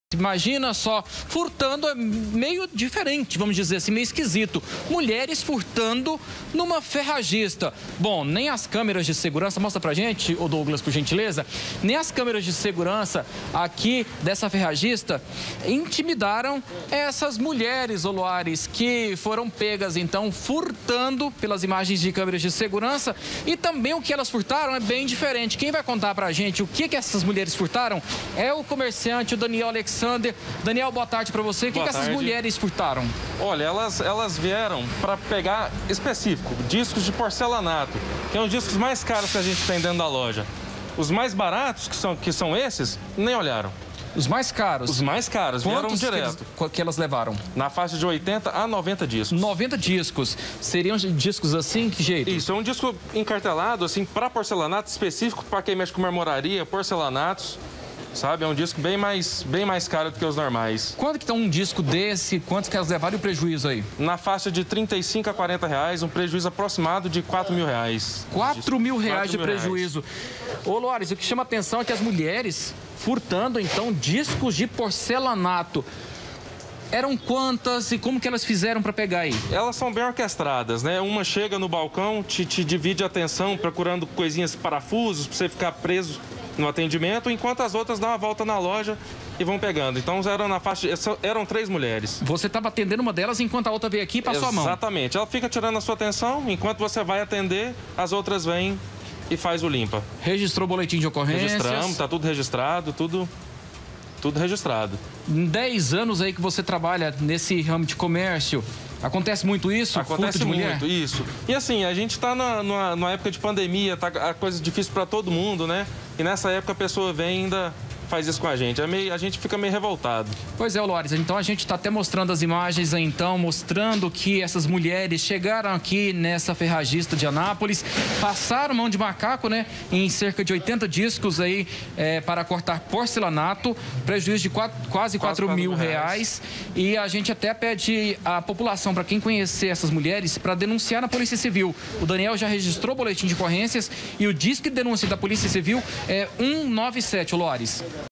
ANÁPOLIS - MULHERES FURTAM OBJETOS EM FERRAGISTA.
MULHERES SÃO FLAGRADAS FURTANDO LOJA FERRAJISTA EM ANÁPOLIS. LEONARDO GONÇALVES.